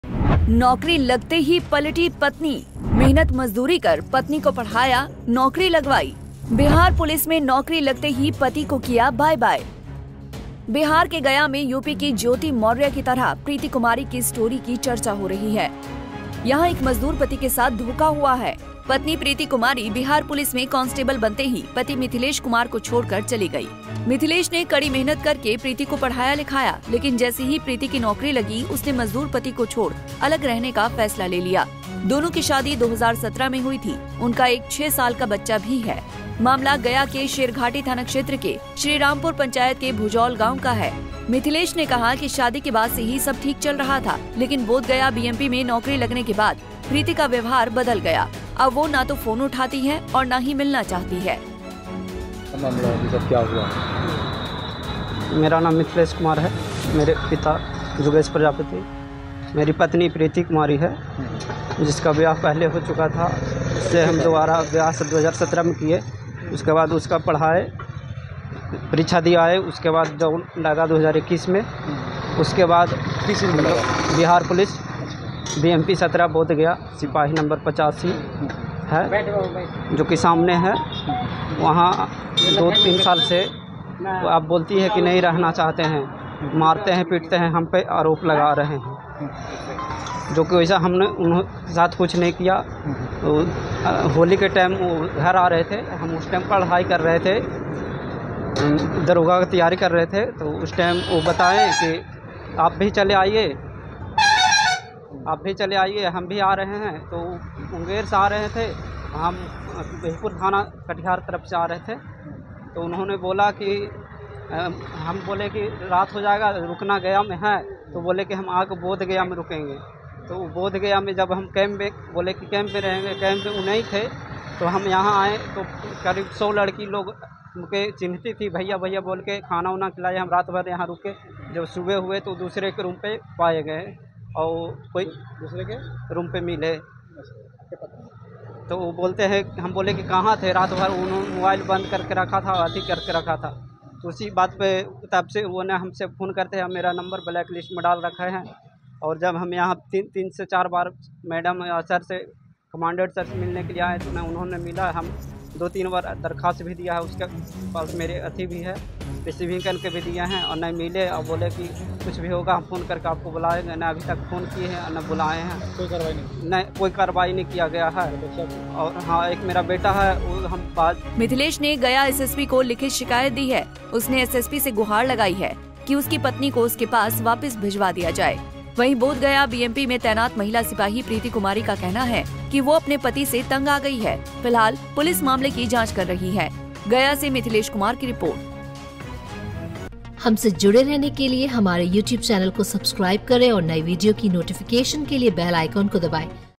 दिन रात मेहनत-मजदूरी कर पत्नी को पढ़ाया, नौकरी लगते ही पति को छोड़ा…
बिहार(Bihar news) के गया (Gaya) में BMP में नौकरी लगते ही पत्नी ने पति को आंखें तरेरनी शुरू कर दी.. वह अपने पति को पहचानने से इनकार कर रही है... न मिल रही है, न बात कर रही, मोबाइल नंबर को ब्लैक लिस्ट में डाल दिया है